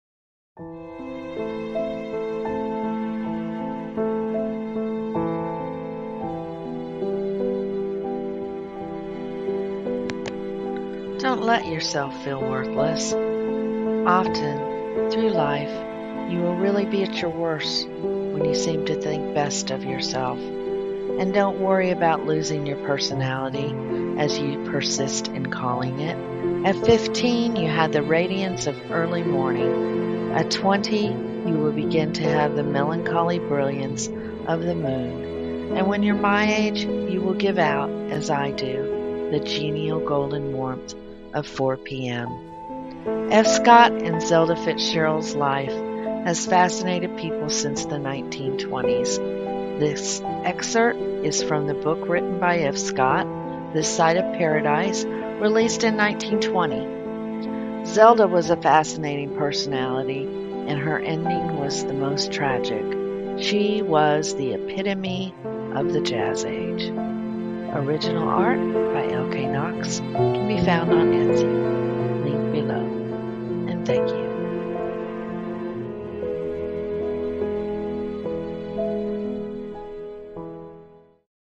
This Side Of Paradise - Quotes of F. Scott Fitzgerald and Zelda
This Side of Paradise was published in 1920, and F. Scott and Zelda became the "people" to watch.  Glamour in the Jazz Age.  Flappers, fun, parties - until it all came crashing down.  This is an excerpt from the book with original art that can be found:
Etsy Shop